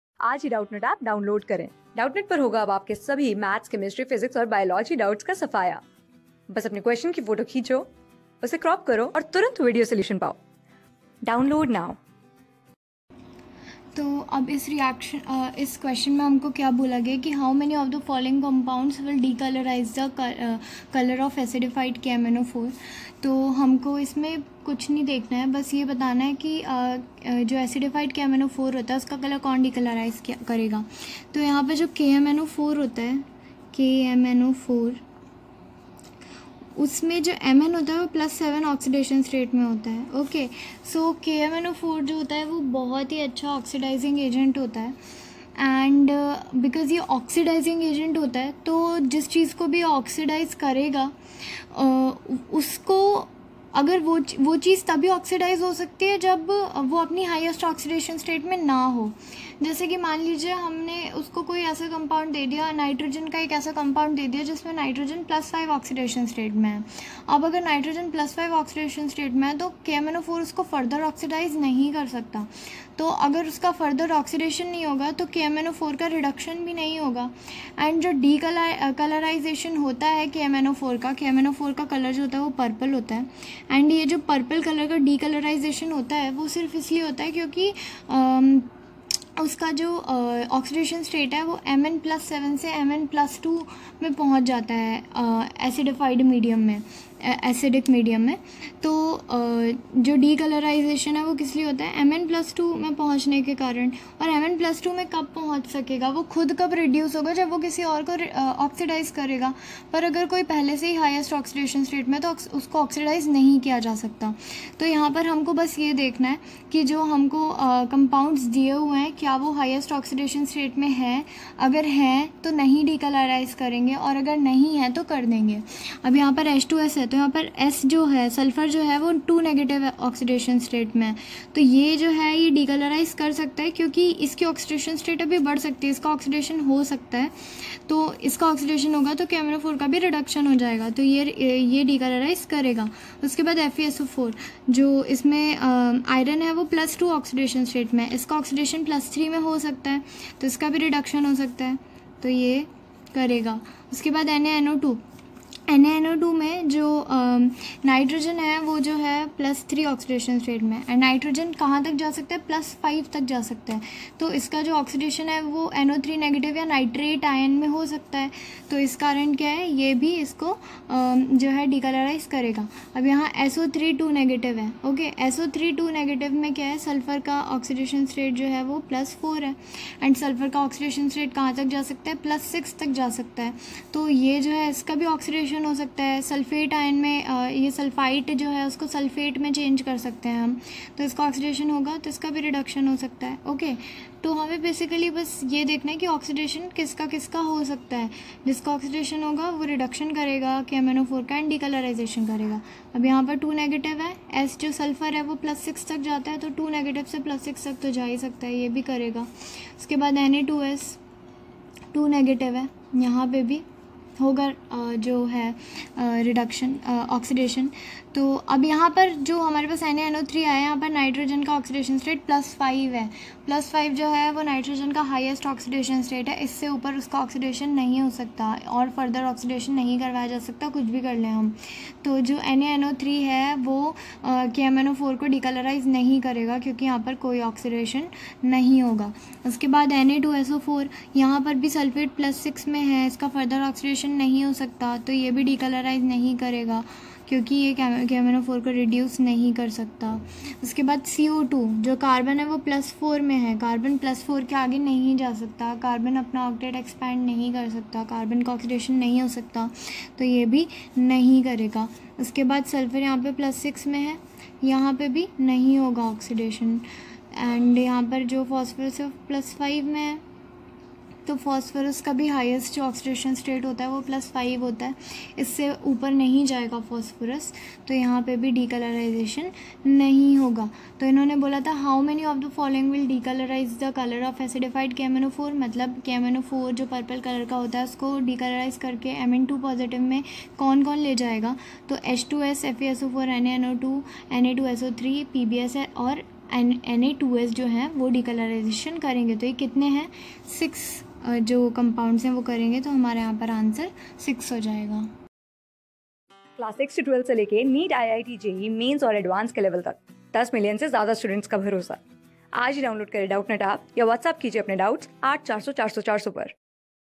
How many of the following compounds will decolourize the colour of acidified ` KMnO_(4)`?
How many of the following compounds will decolourize the colour of acidified ` KMnO_(4)`? `{:(H_(2)S,FeSO_(4),NaNO_(2)),(Na_(2)SO_(3),PbS,Na_(2)S),(NaNO_(3),Na_(2)SO_(4),CO_(2)),(SO_(3),P_(2)O_(5),):}`